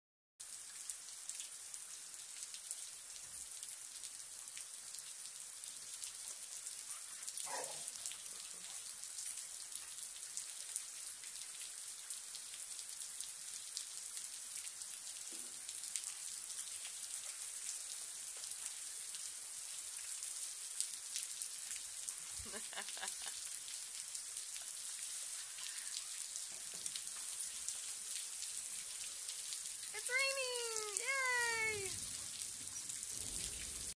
Vader loving the summer rainfall!
Vader playing around in the rain...he loves his water